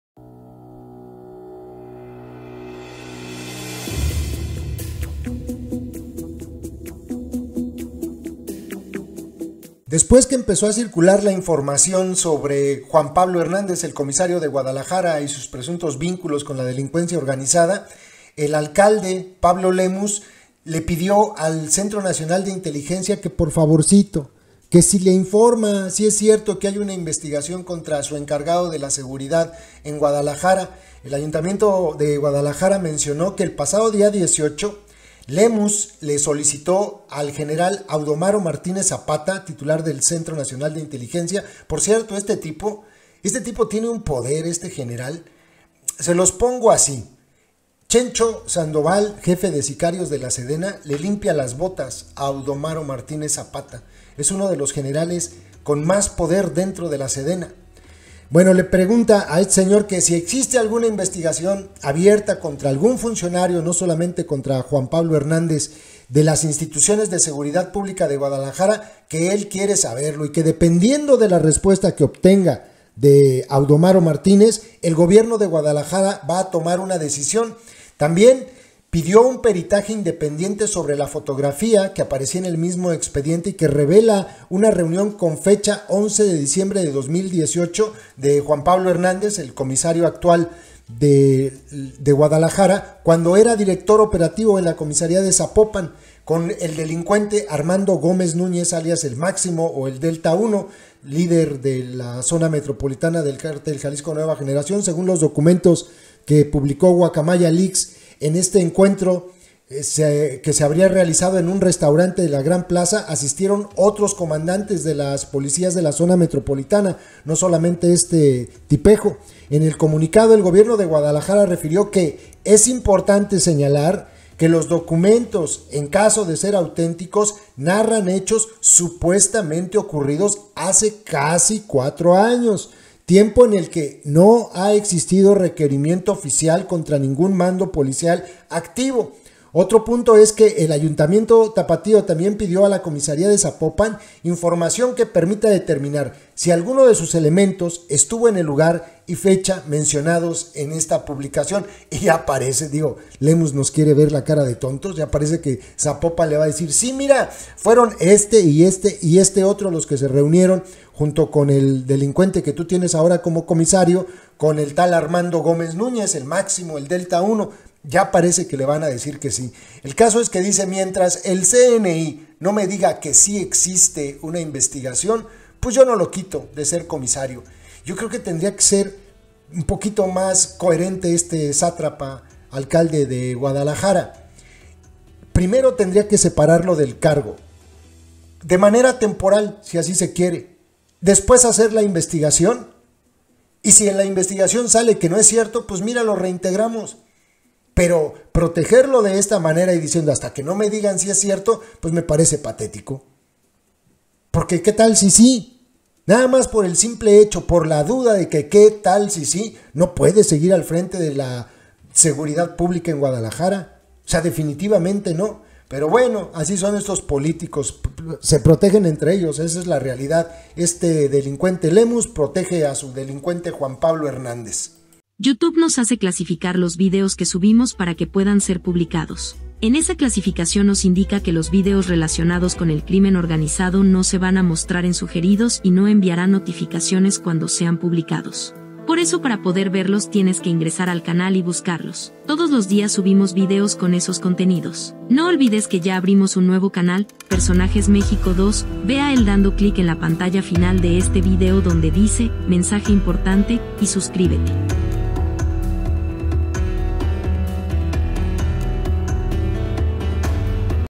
Alcalde de Guadalajara pide a Audomaro Martínez que le informe si su comisario es bueno o malo
Pablo Lemus, alcalde de Guadalajara pidió al General Audomaro Martínez un informe sobre su comisario, Juan Pablo Hernández, tras aparecer en los archivos filtrados de la Sedena. Dice que le urge para poder tomar una decisión sobre si lo despide o sigue siendo su confiable comisario.